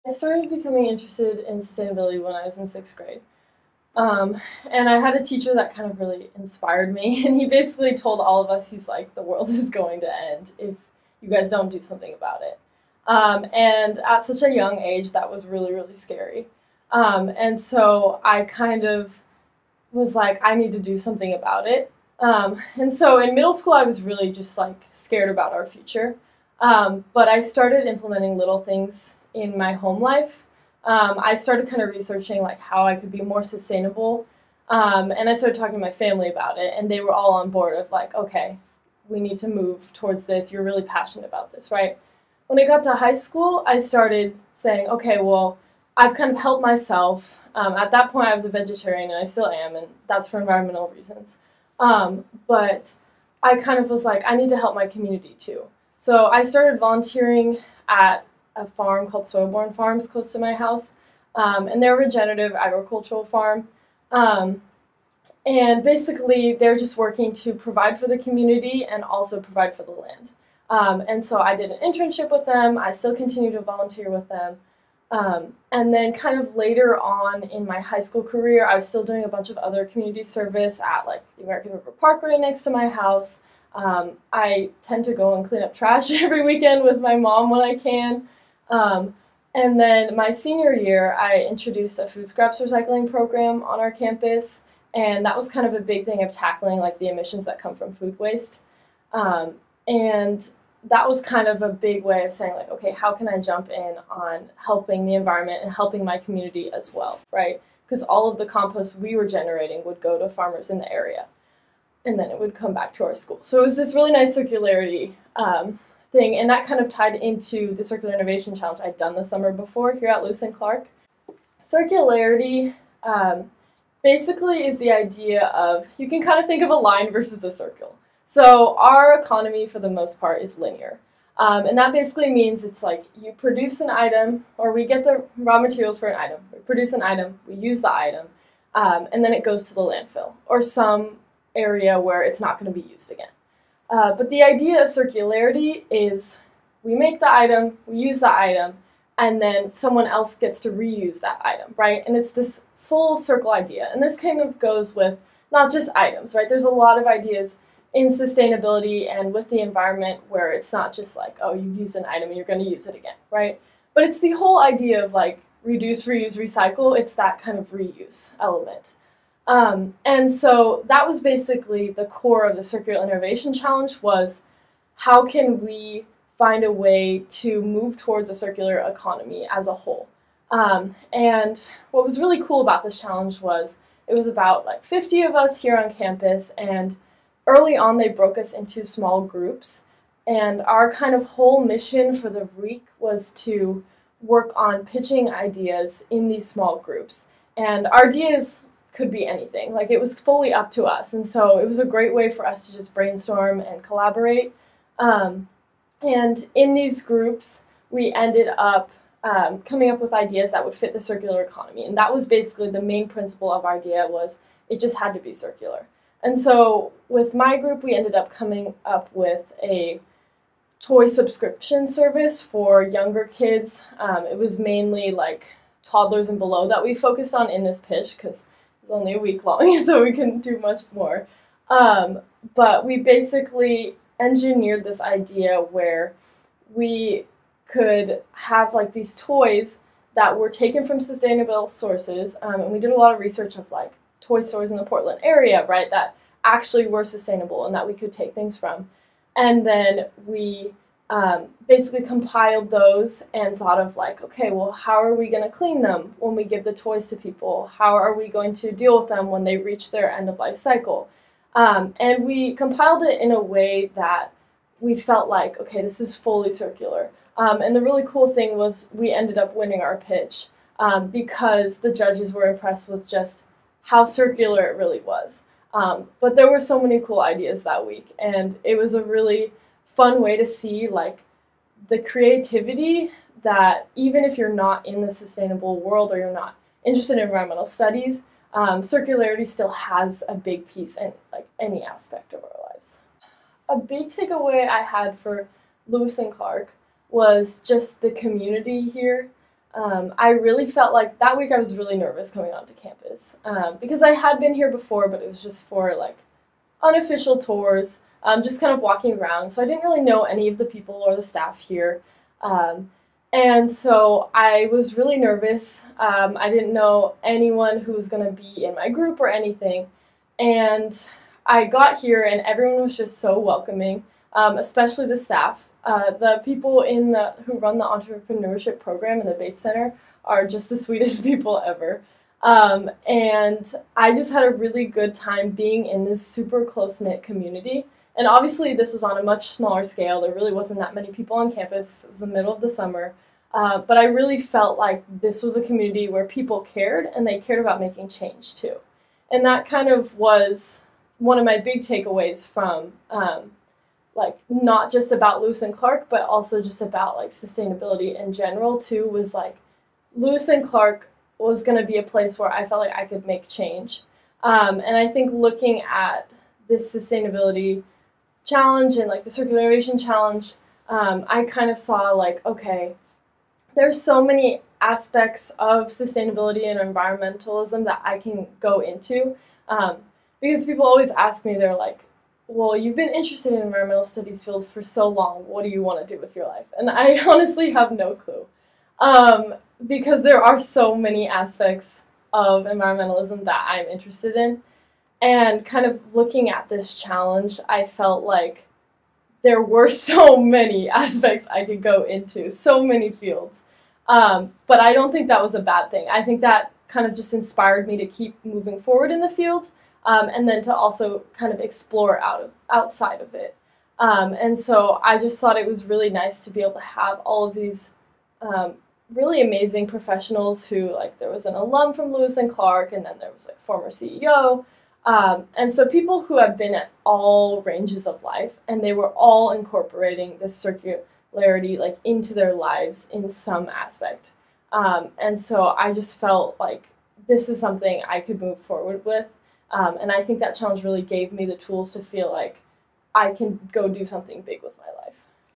Pio Spotlight: Emilie Thorsen (Volleyball) Attending the L&C Circular Innovation Challenge
Lewis & Clark volleyball first year Emilie Thorsen has had a passion for sustainability and finding ways to save the environment since she was in the sixth grade. She describes why she originally became interested in sustainability and how participating in the Lewis & Clark Circular Innovation Challenge at Lewis & Clark impacted her in her interests in sustainability and coming to L&C.